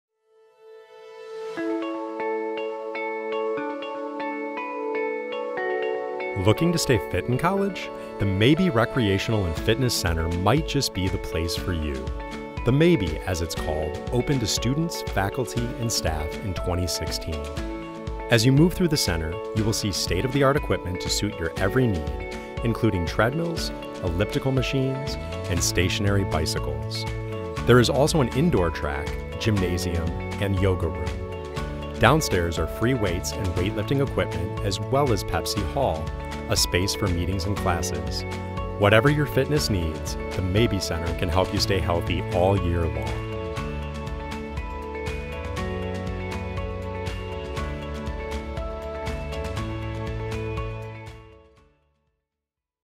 J.E. & L.E. Mabee Recreation and Wellness Center
Take a 360º tour of the Mabee Center.